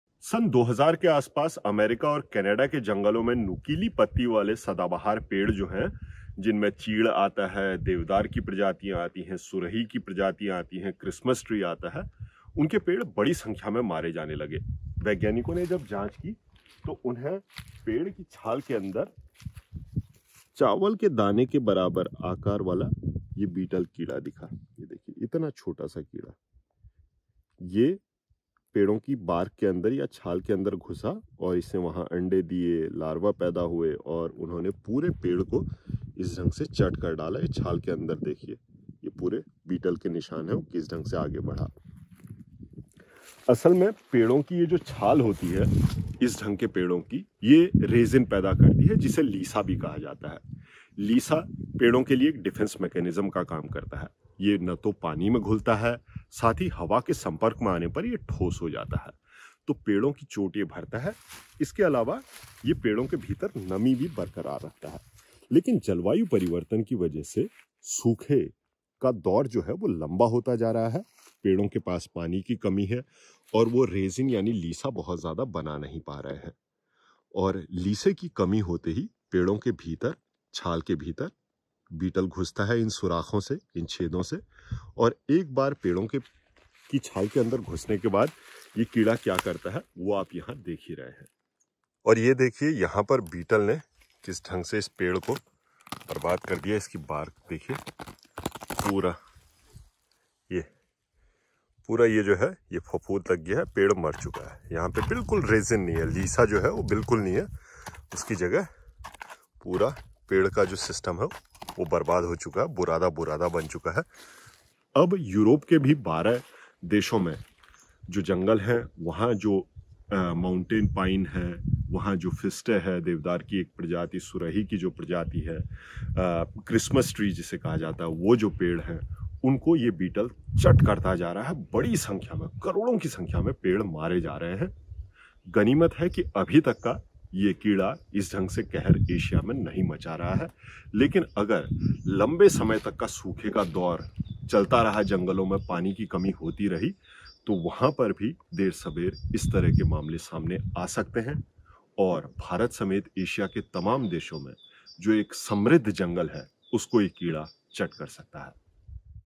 Forest killer insect | Pin N Post
चावल के बराबर आकार के एक कीट ने यूरोप और अमेरिका के जंगलों में तबाही मचाई हुई है. बीटल (Beetle) नामके इस कीट ने बीते 20 साल में करोड़ों पेड़ मार दिए हैं. एक नुकीली पत्तियों वाले सदाबहार पेड़ों को निशाना बना रहा है. जलवायु परिवर्तन के कारण यह सामान्य प्रक्रिया बड़ी पर्यावरणीय त्रासदी में बदल चुकी है.

Follow Pin N Post:
Youtube पर